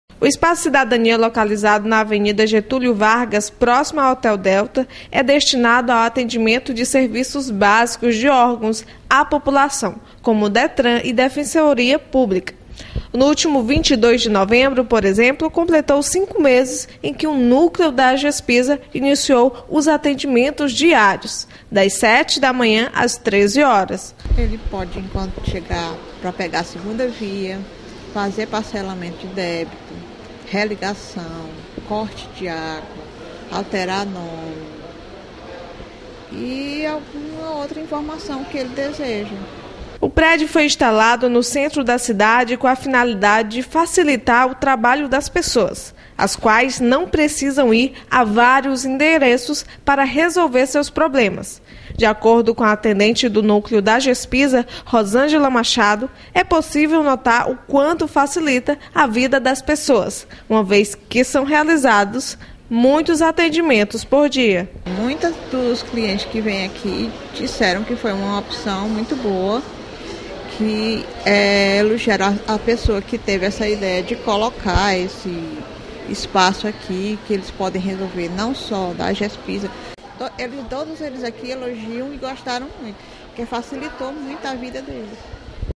AGESPISA ESPAÇO E CIDADANIA
Núcleo de atendimento da AGESPISA no Espaço e Cidadania completa cinco meses de funcionamentos e identifica grande melhoria no atendimento.